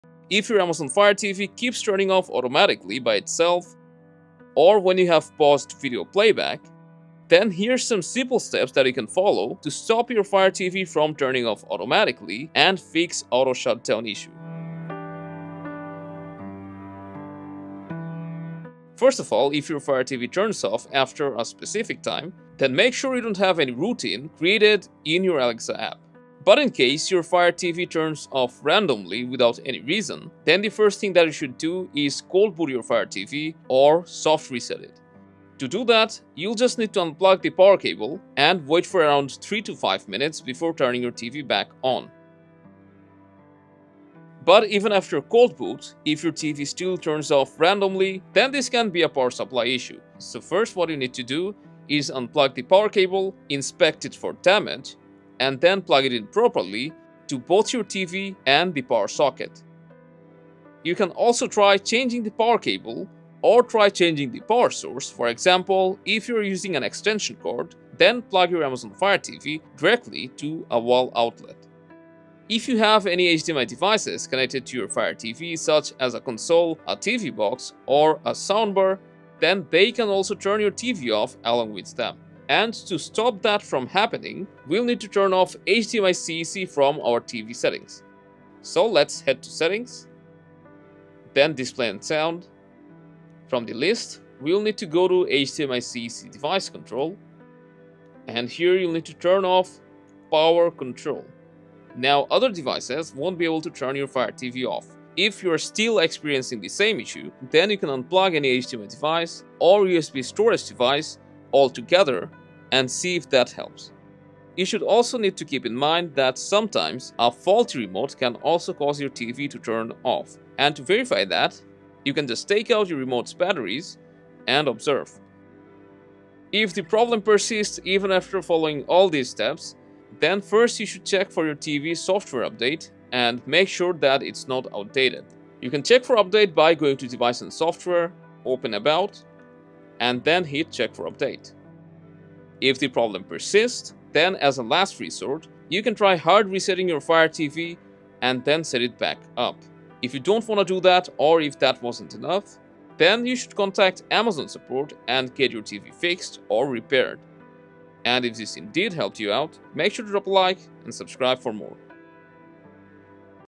Amazon Fire TV Keeps Turning Off By Itself? – How to Fix Auto Power Off!
Does your Amazon 4K Fire Television running on Fire OS keep turning off automatically by itself at random times? Do you need help fixing auto shutdown problems on your Amazon Fire TV to prevent it from randomly going to sleep? No worries—the Fix369 team has an easy troubleshooting guide.

Let’s have a look at 6 straightforward solutions to stop Amazon Fire TV 4K from powering off issues effectively once and for all.

0:00 Intro: Amazon Fire TV Turning OFF Automatically
0:18 Solution 1: Check Routine and Cold Boot
0:48 Solution 2: Check the Power Cord/ source
1:12 Solution 3: Disable HDMI-CEC
1:50 Solution 4: Remove all connected Accessories and check the Remote
2:12 Solution 5: Software Update
2:27 Solution 6: Hard Reset and Contact Amazon Support